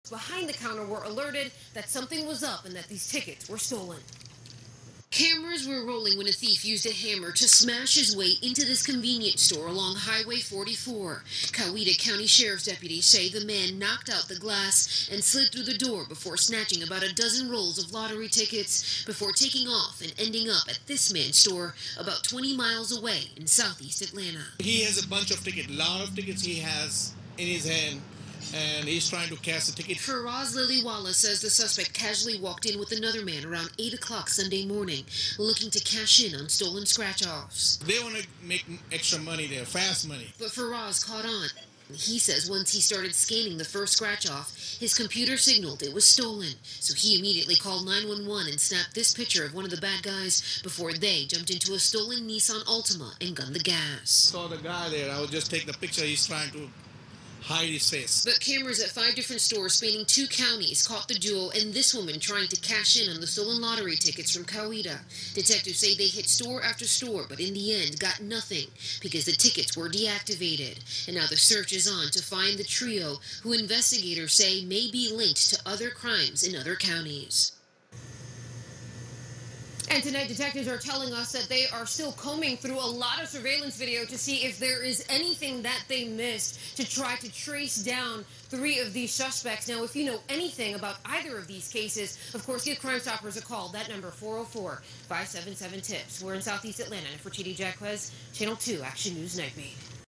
Let's see what happens when I steal scratchers from a gas station & try to cash them in........
Let's see what happens when I steal scratchers from a gas stations & try to cash them in.